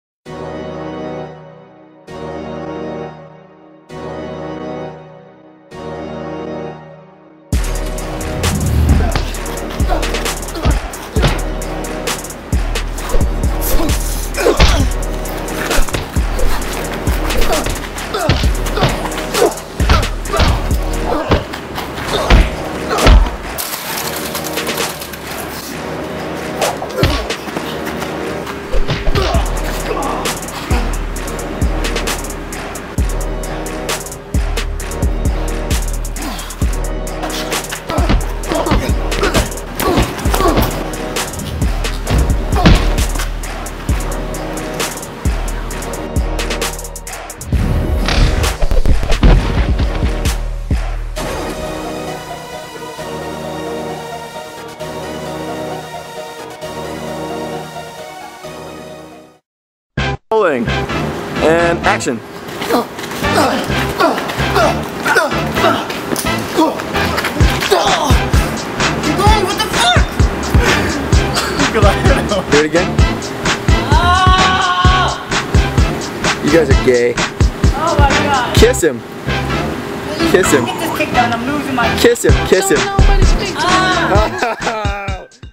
Tony Vo vs Eric Van Arsdale
Made a lil fight to use for our stunt reels!! Watch in 720p! =)